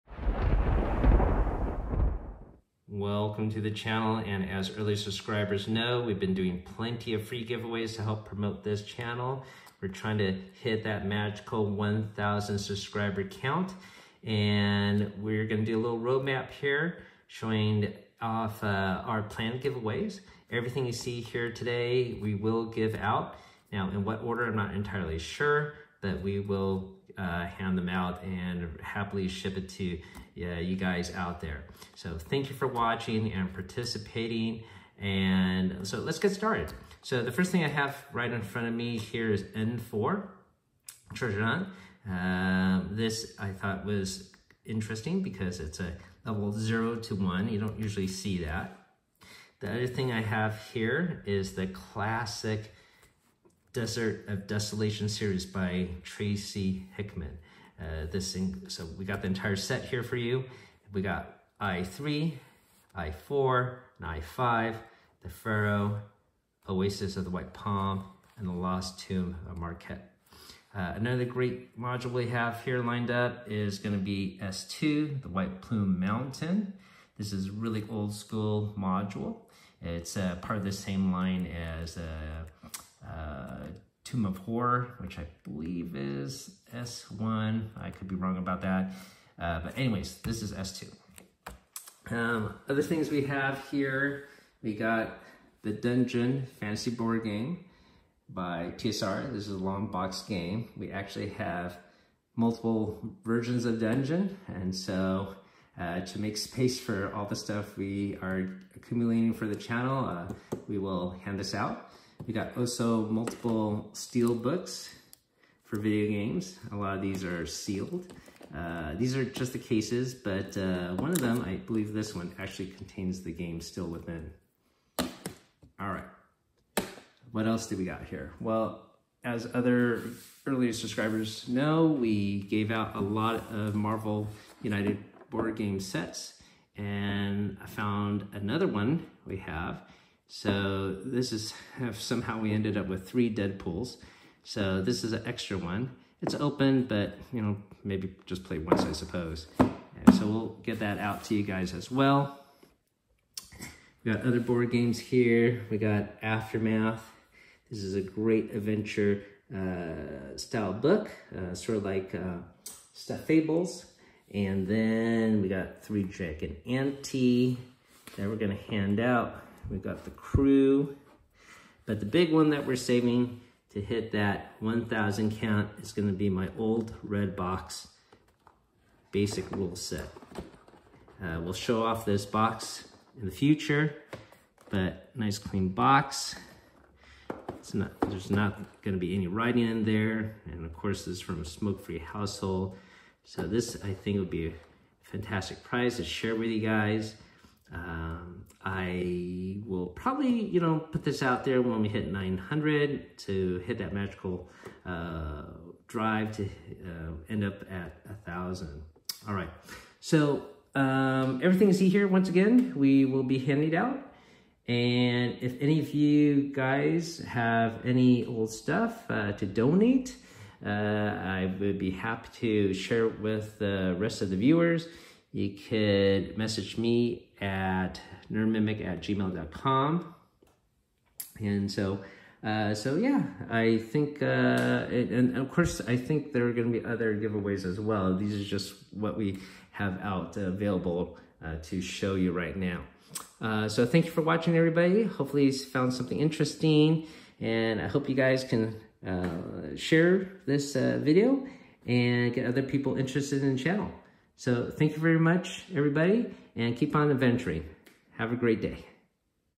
FREE Giveaways Roadmap - The path towards 1000 subs! Dungeons and Dragons, Board Games, Video Games
Only some of the stuff we have lined up to promote the channel.  Most are duplicates from various means that we rather giveaway than sell on EBay.  We hope they will have a happier home this way.

As always, I will mail the prizes at my own cost anywhere within US only, to yourself or a friend/family within the US.  And winners have 7 days to claim the prizes.

Big shout out to Rooftop Games (link below) for providing great miniatures and other D&D materials over the past year.  Thanks Chris!  Let him know I sent you!